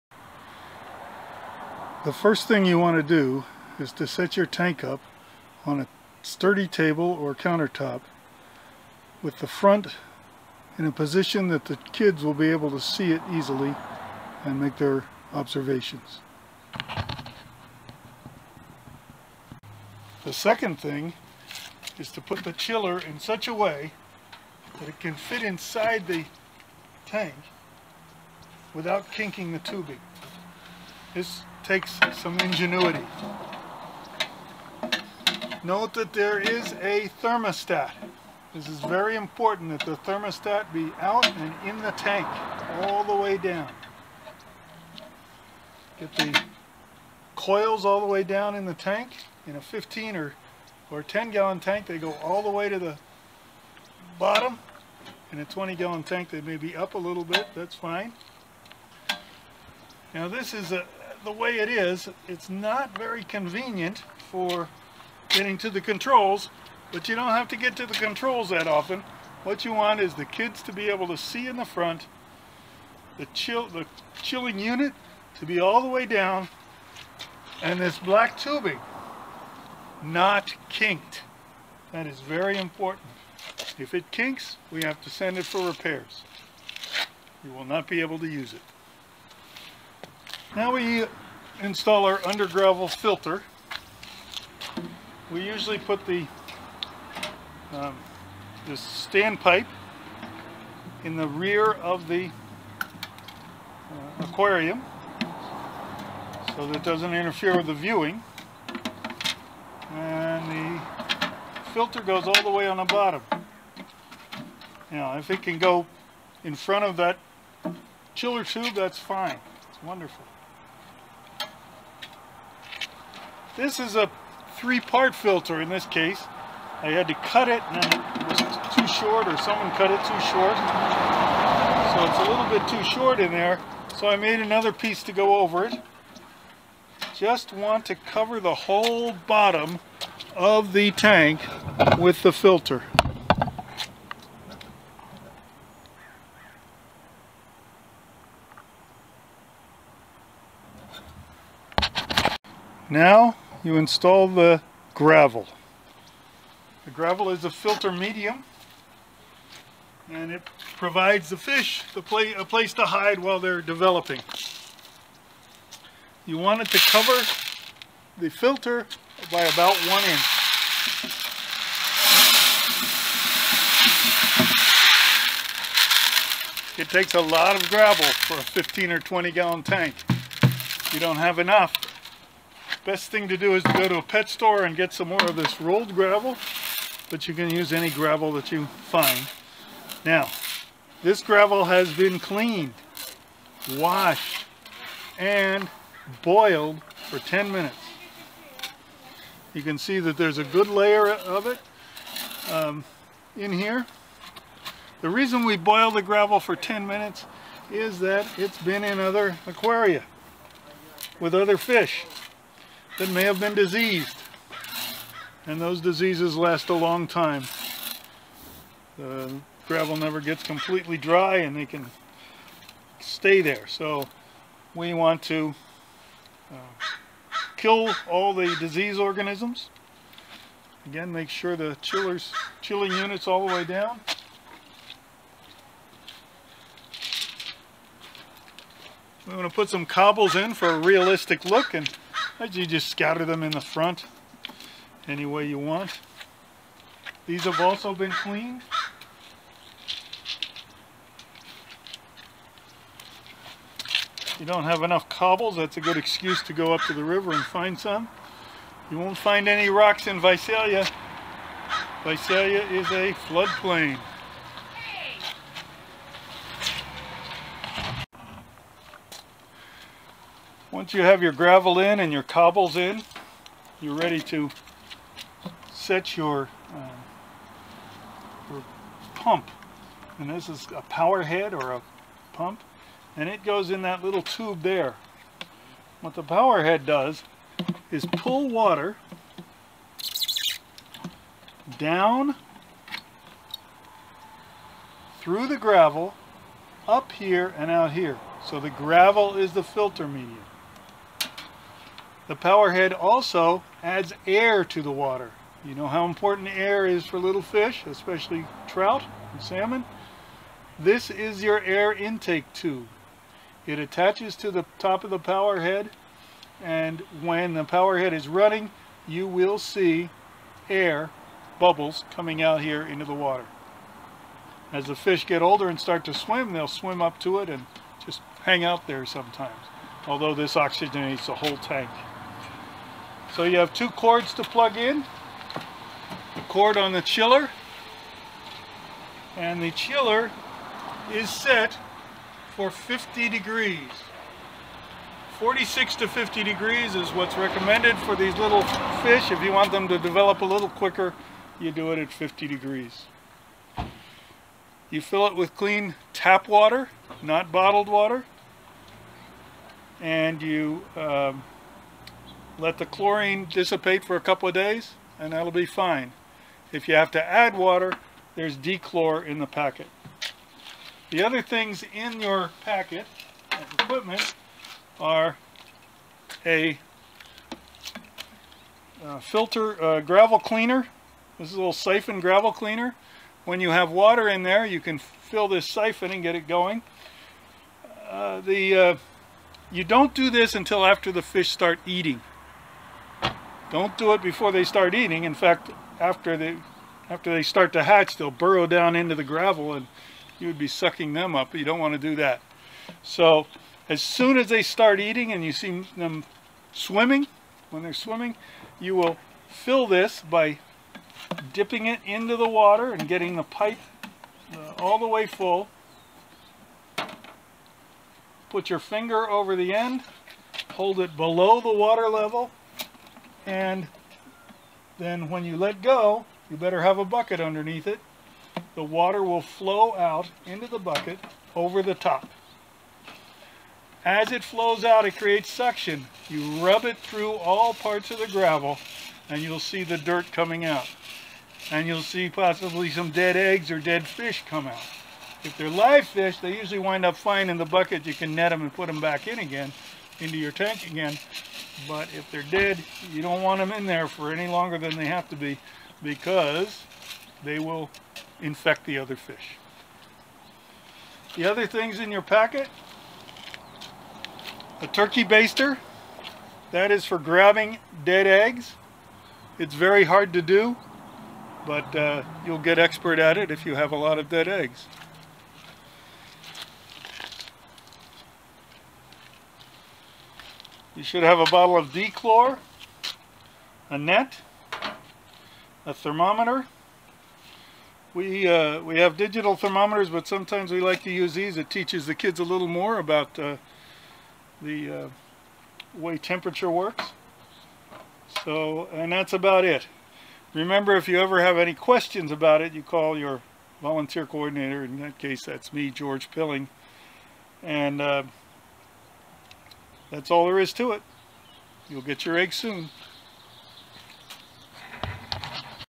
Trout in the classroom aquarium setup
How to set up your Trout in the classroom equipment using an immersion type chiller unit. Video 2.